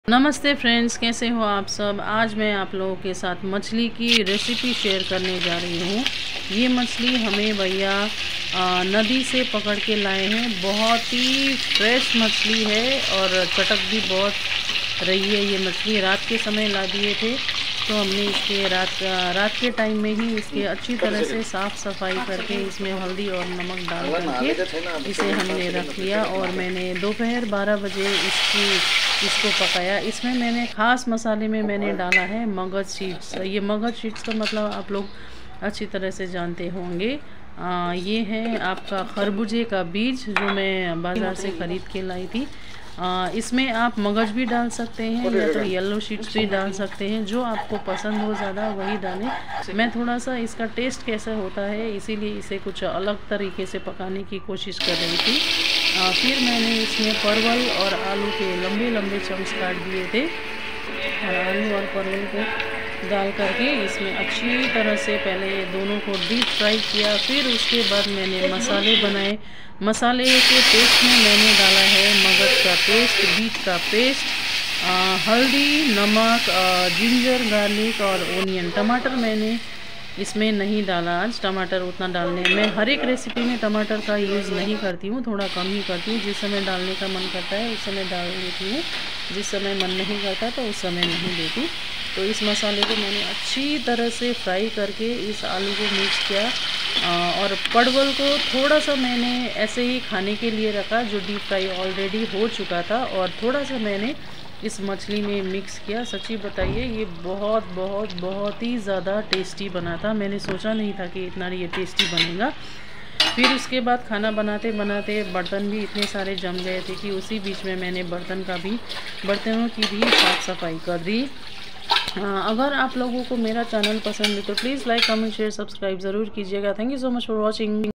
gudmorning🌄🙏namestey friend's fresh#fish#rohufishcurry#food#youtubeshorts #@ushabiswakarma7696
"life with usha"
fresh rohu fish curry in my own way with magaj seeds, beetroot paste, chilly, onion, ginger garlic paste, haldi salt and a pinch of red chilli powder ."try jarur karey"